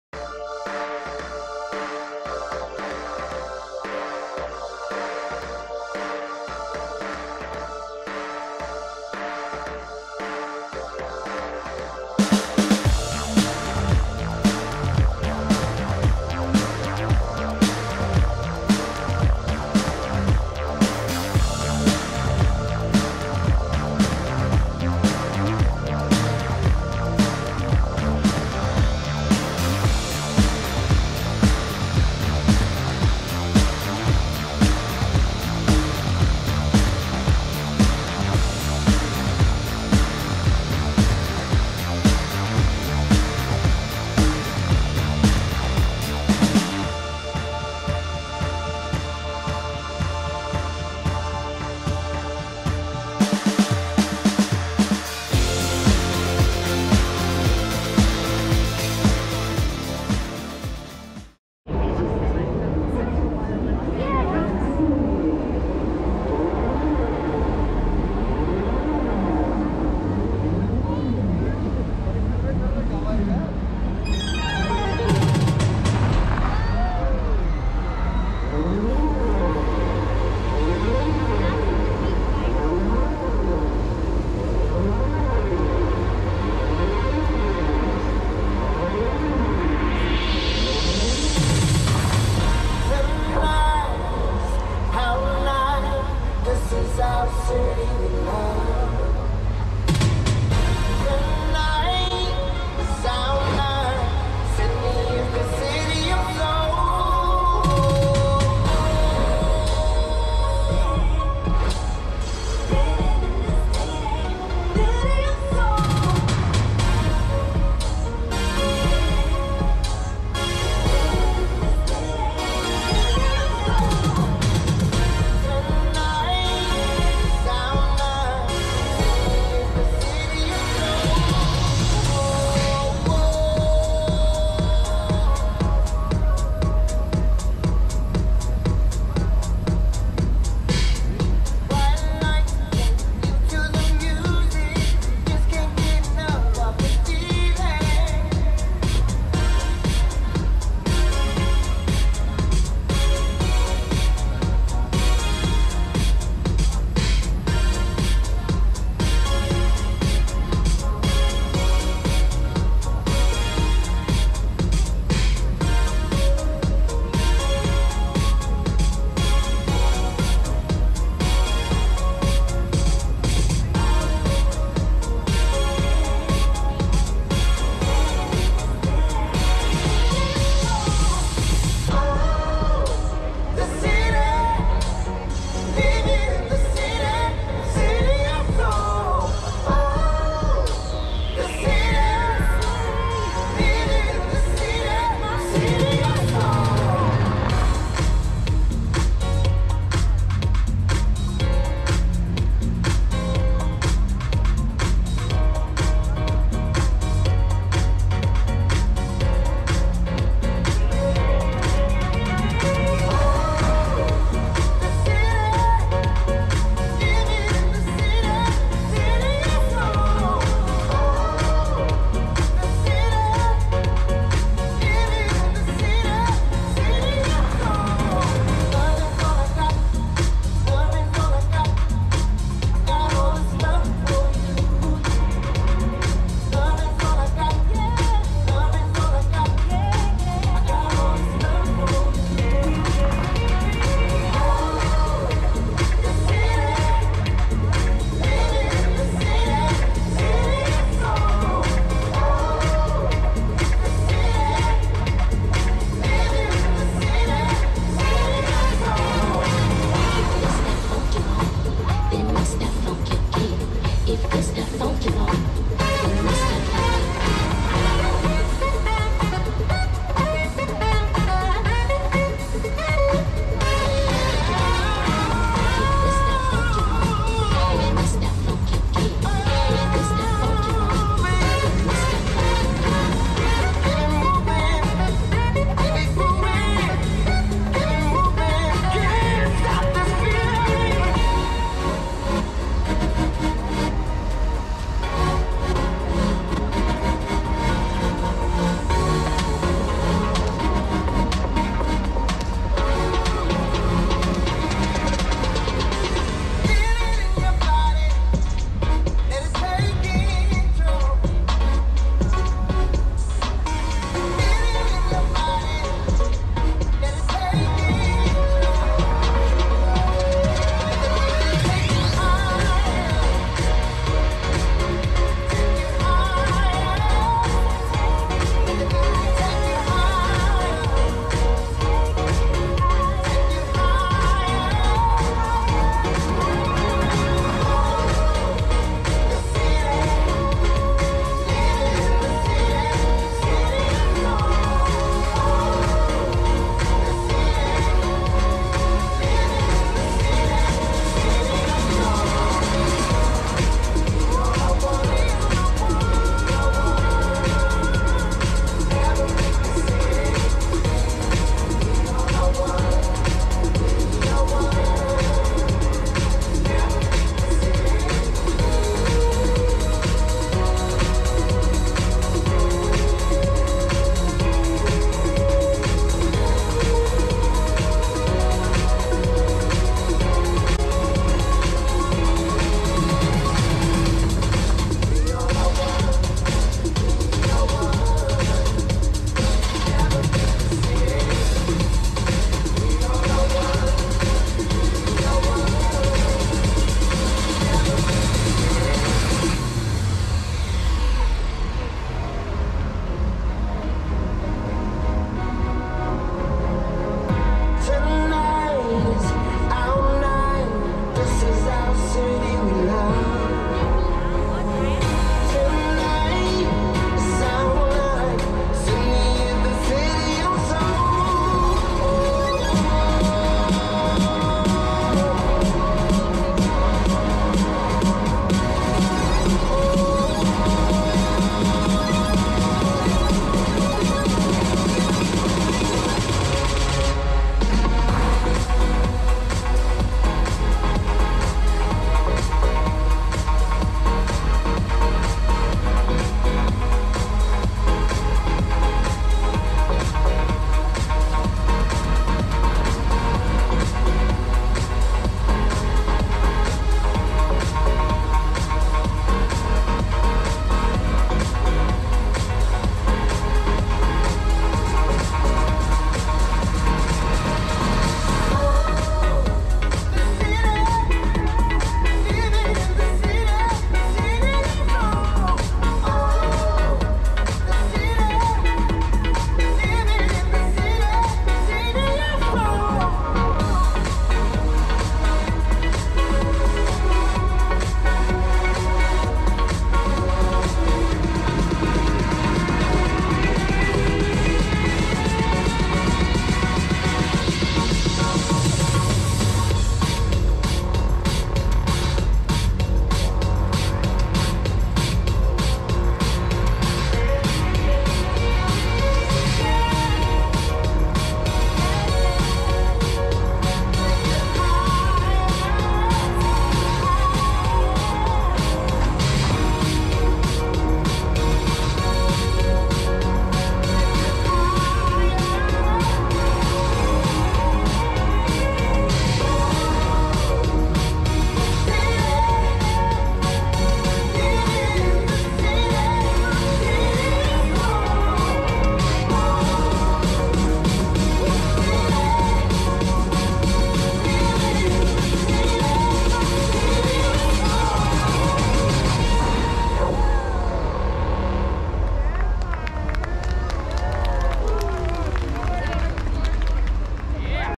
Vivid Sydney Part III (Darling Harbour)
Vivid Sydney is the worlds largest festival of light. This year, the festival boasts the longest continuous Light Walk in the history of the event from the Sydney Opera House to central station.

We always have fun filming new things for you guys and showing you places to go.